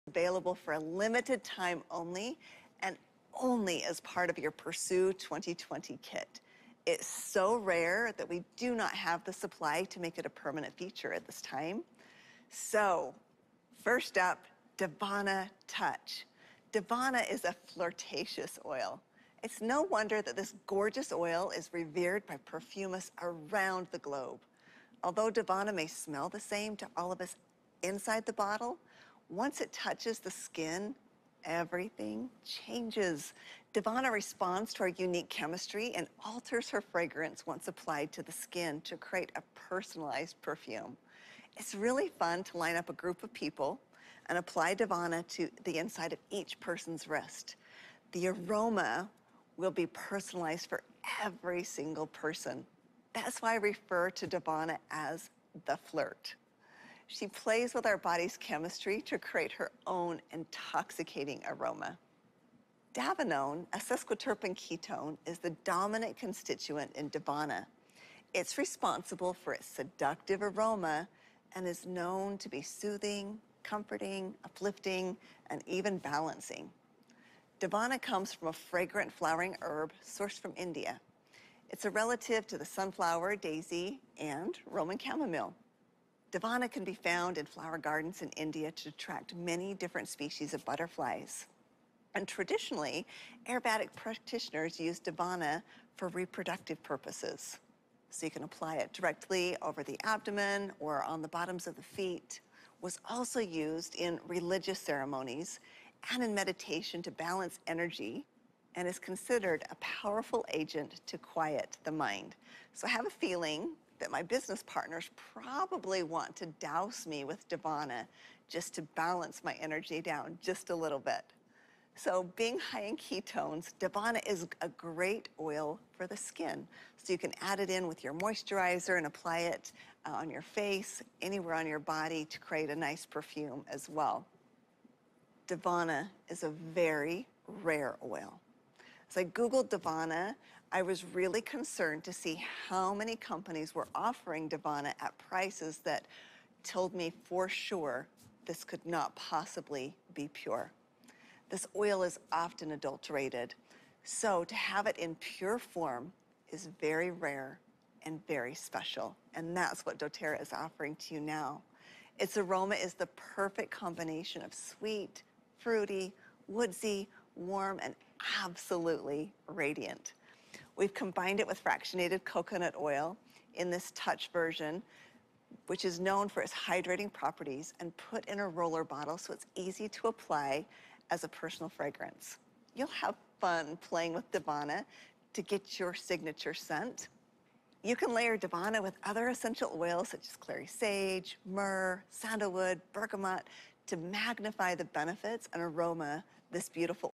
『誰說化學不重要 ! 』教你秒懂化學 讓你使用精油不用死背燒破腦 簡單靈活運用精油 印蒿精油介紹
這方面的知識是我們「Double 達倍團隊」最引以為傲的
強項，不管你是精油小白還是芳療師
Whatever ~
了解精油化學我們都能讓你
將精油簡單掌握在手 實踐精油生活！

以下是
2020台灣年會新品之一 ，印蒿精油的介紹影片
資料同步美國新品